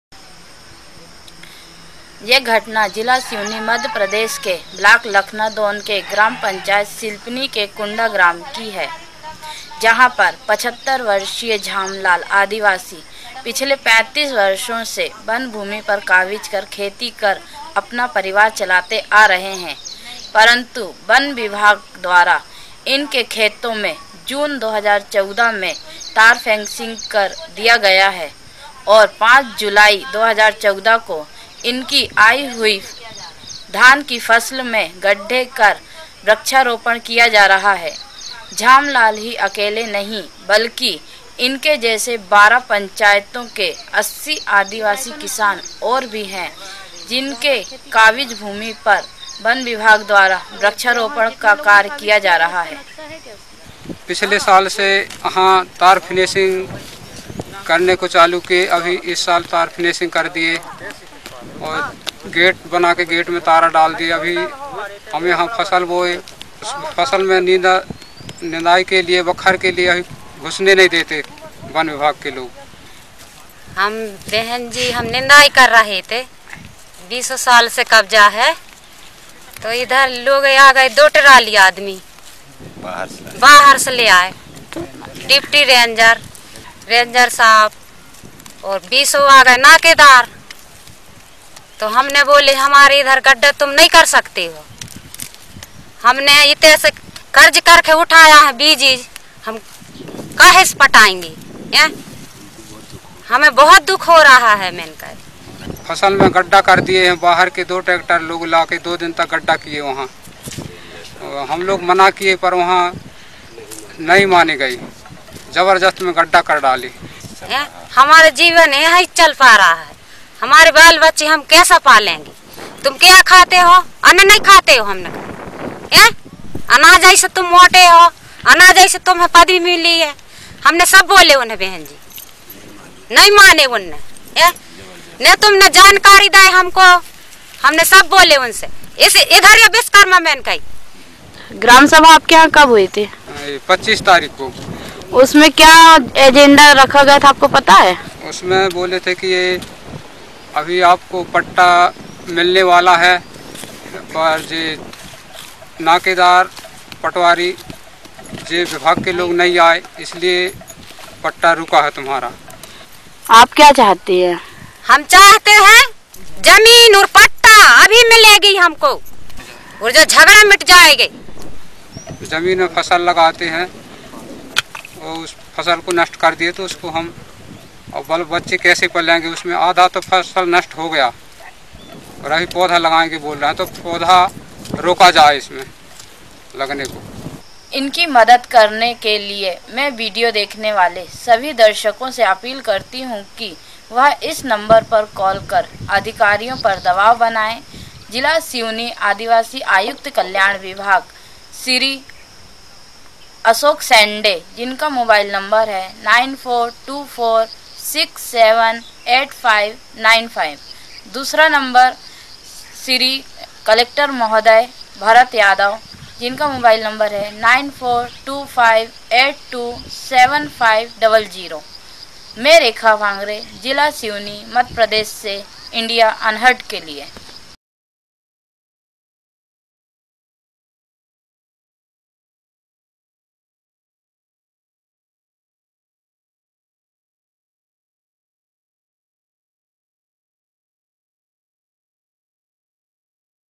The War Between Adivasis and the Forest Department in MP - Video Volunteer Rekha Bhangre Reports
This video was made by Video Volunteers Community Correspondent Rekha Bhangre